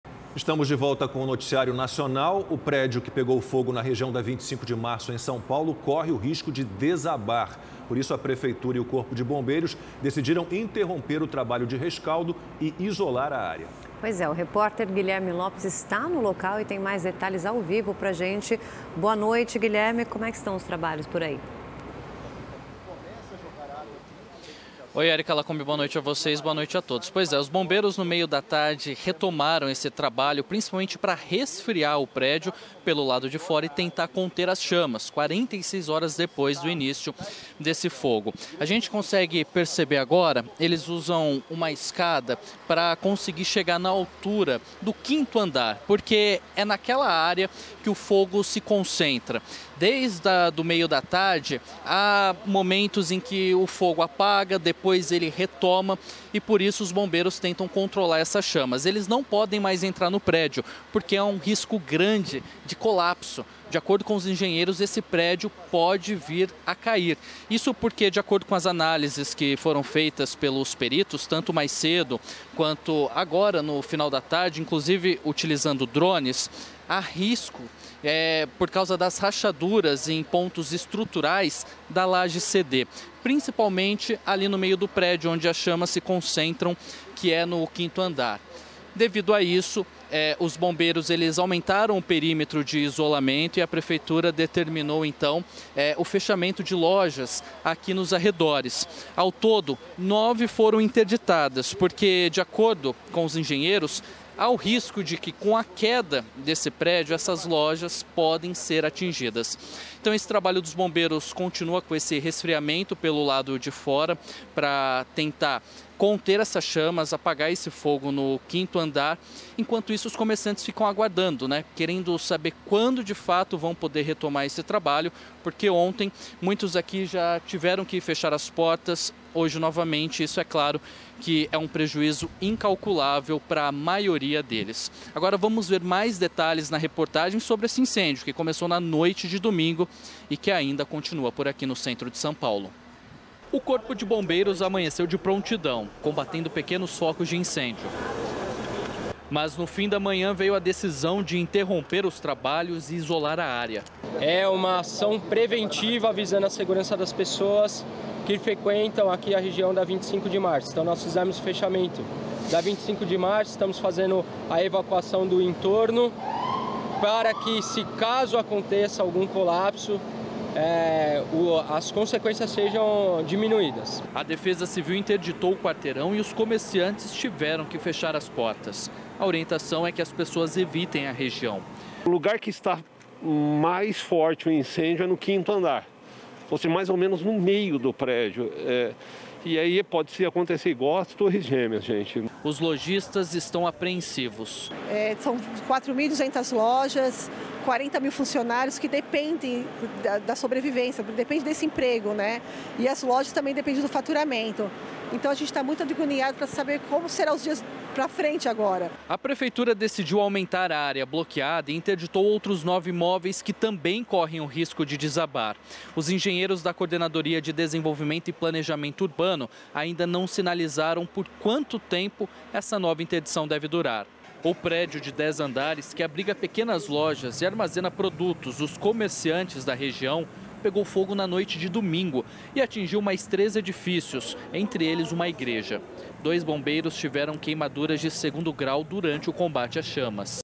Prédio que pegou fogo no centro de São Paulo pode desabar, afirma Corpo de Bombeiros
O edifício que pegou fogo na região da 25 de março, em São Paulo, corre o risco de desabar. Sendo assim, a prefeitura e o Corpo de Bombeiros decidiram interromper o trabalho de rescaldo e isolar a área. Saiba mais na reportagem do